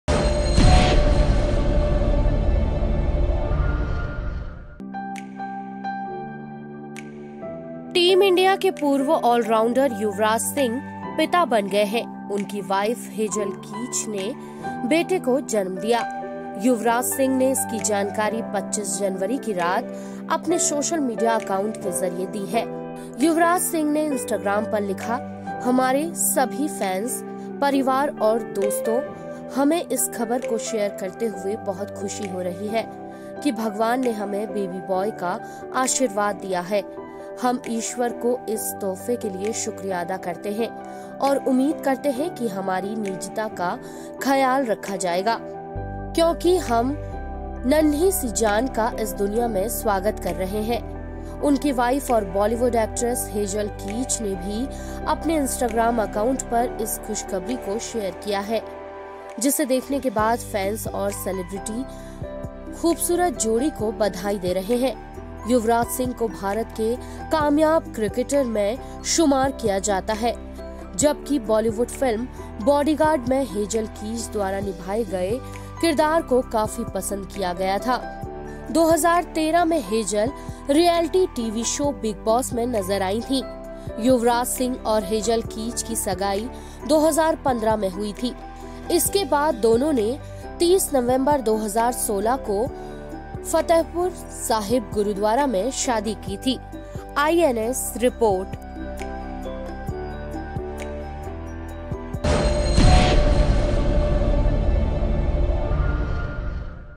पूर्व स्टार क्रिकेटर युवराज सिंह और हेजल कीच सिंह बने माता पिता
टीम इंडिया के पूर्व स्टार ऑलराउंडर युवराज सिंह  पिता बन गए हैं।  उनकी वाइफ हेजल कीच ने बेटे को जन्म दिया ,  युवराज सिंह  ने इसकी जानकारी 25 जनवरी की रात अपने सोशल मीडिया अकाउंट के जरिए दी ।